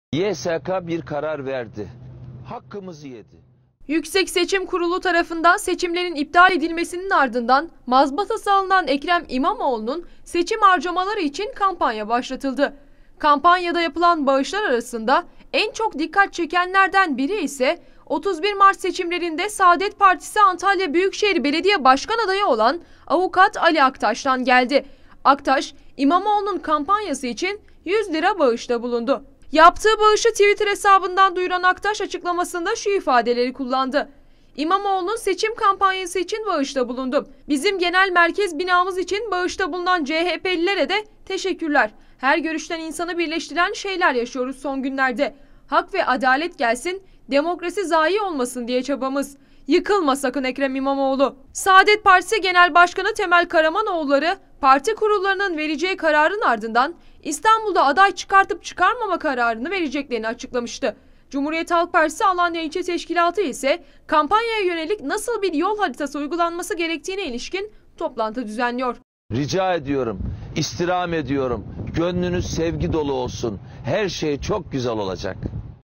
SP ADAYINDAN İMAMOĞLU’NA DESTEK
Saadet Partisi’nin 31 Mart yerel seçimlerinde Antalya Büyükşehir Belediye Başkan Adayı olan Ali Aktaş, İstanbul Büyükşehir Belediye Başkan Adayı Ekrem İmamoğlu'nun seçim kampanyasına 100 bin TL bağışta bulundu. Ali Aktaş, 100 bin liralık bağışta bulunduğunu gösteren görseli de Twitter'da paylaştı.